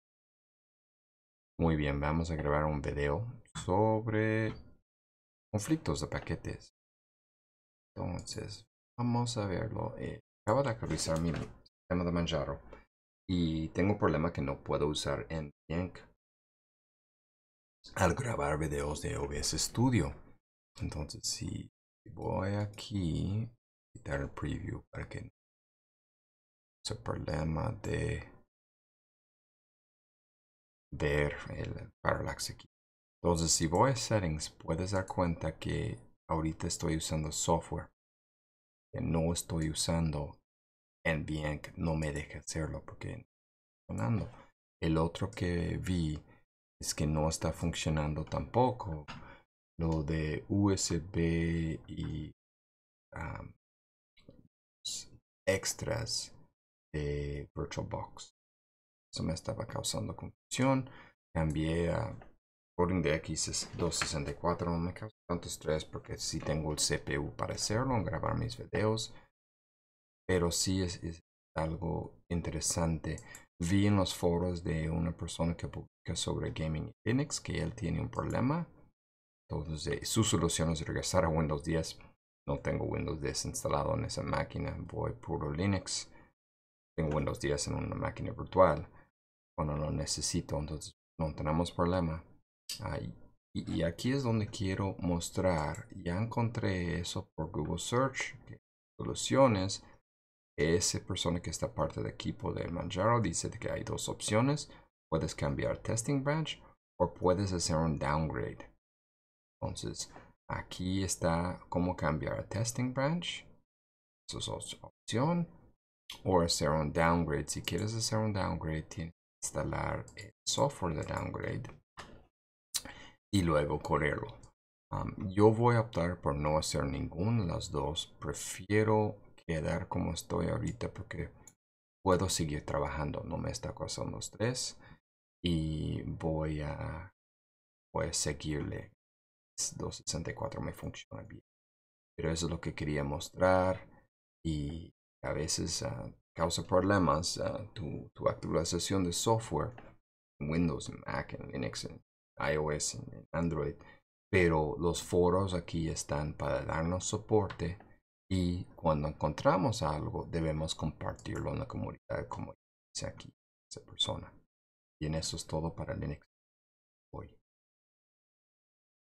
LinuxTips - Conflictos al Actualizar Manjaro (NVENC OBS Studio)
El otro día después de actualizar mi Manjaro, noté problemas en OBS Studio y Virtualbox. No puedo usar NVENC para OBS y problemas de los extensions de Oracle para Virtualbox. Aquí documentando como buscar solución y compartir con la comunidad.

LinuxTips es una serie de videos cortos en español sobre el uso de Linux en la vida diaria.
Publicando cada martes y jueves